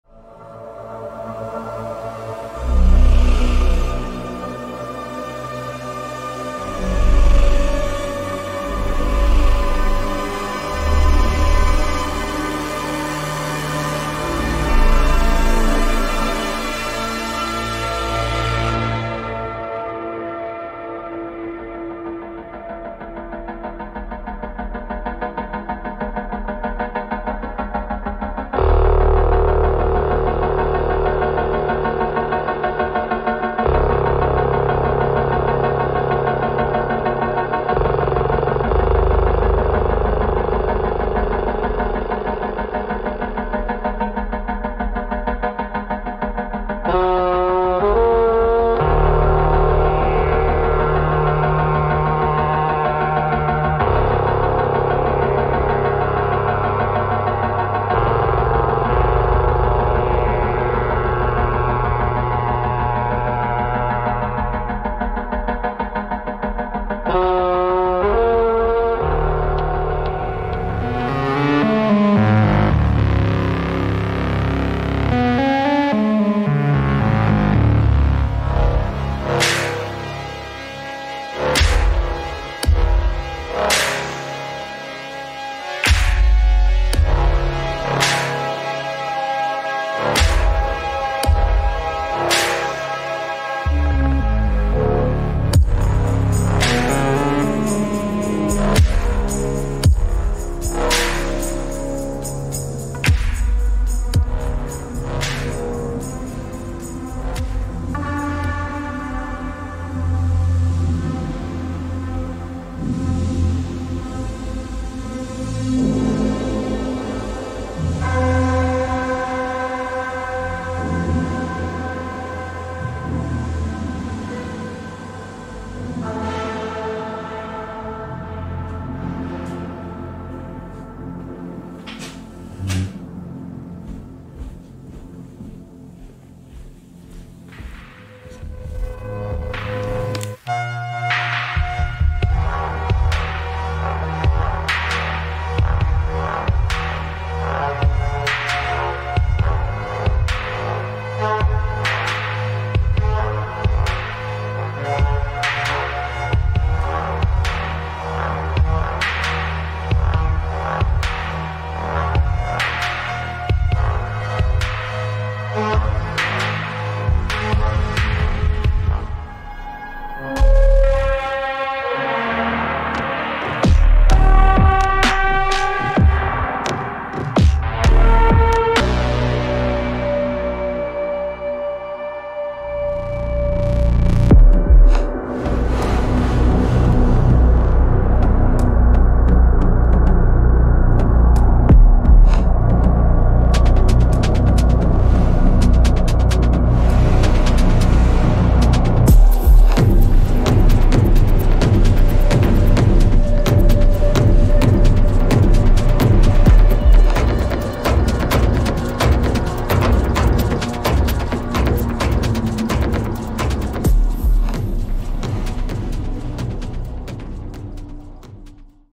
LORN VISUALIZER DEMO | 4K
A short compilation video of different tracks run through a visualizer application I build for Lorn